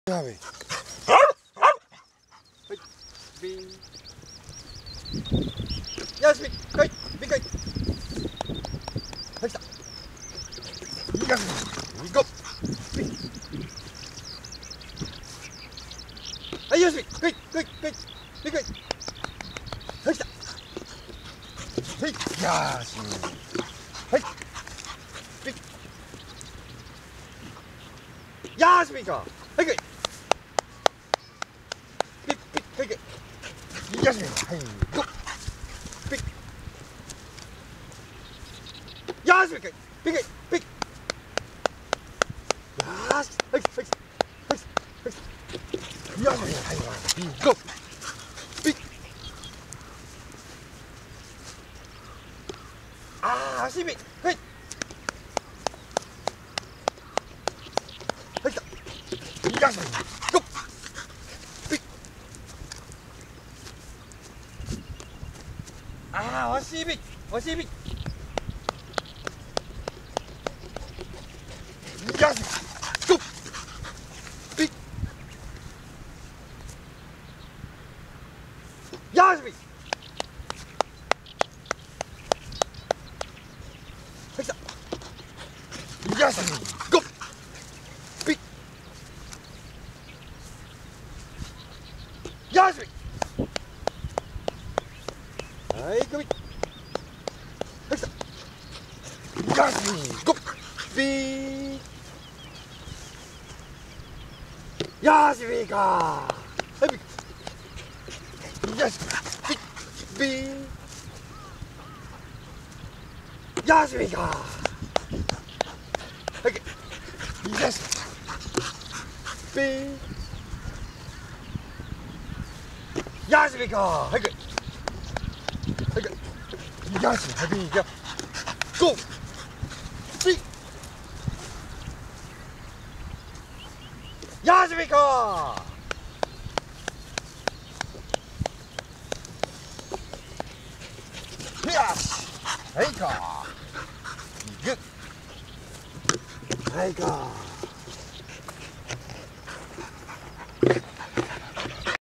ラブラドールレトリーバー／ディスクの練習
フィールド系ラブラドールレトリーバーのディスクの練習です。